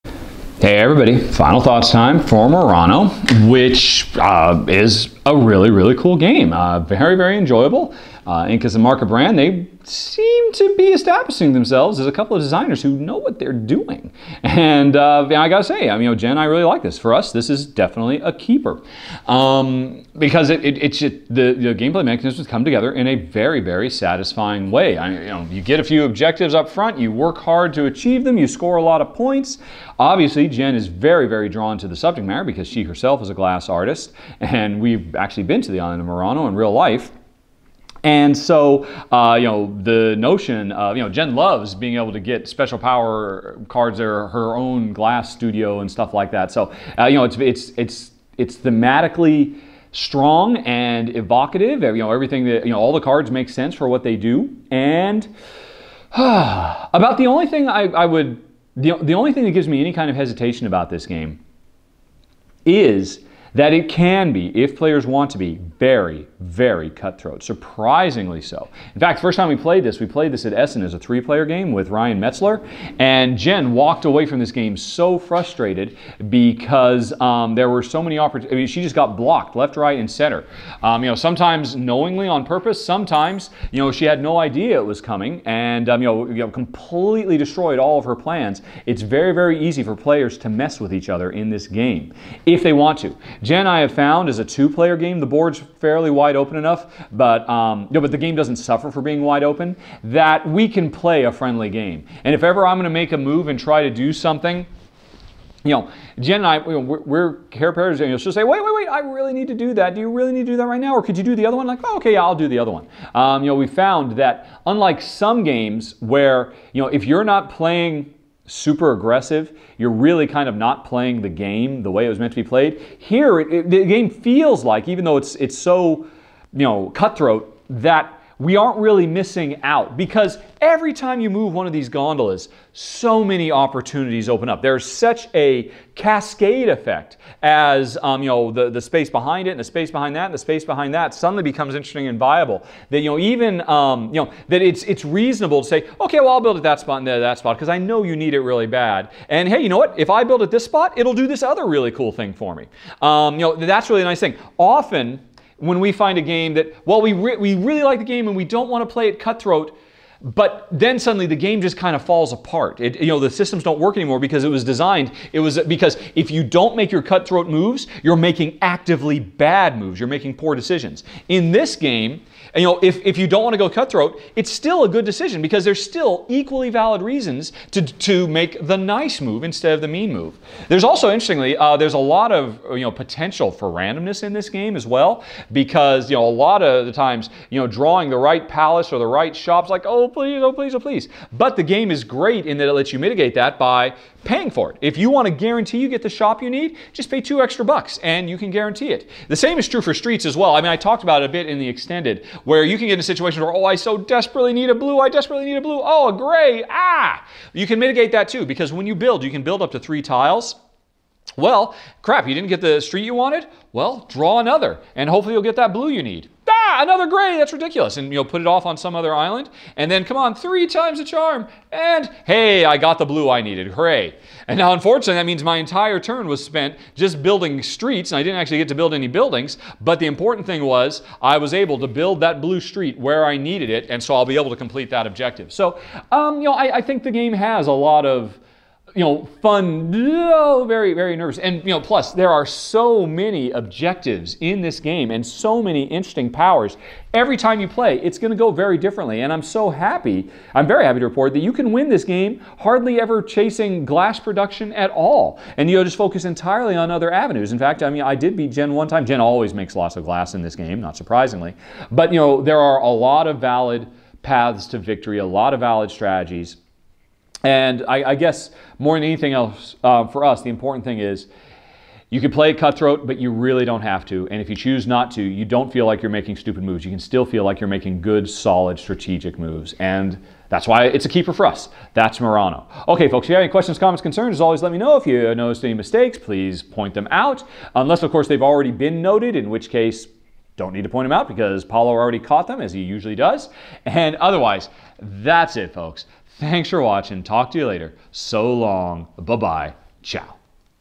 Murano | Rahdo's Final Thoughts
A video outlining gameplay for the boardgame Murano.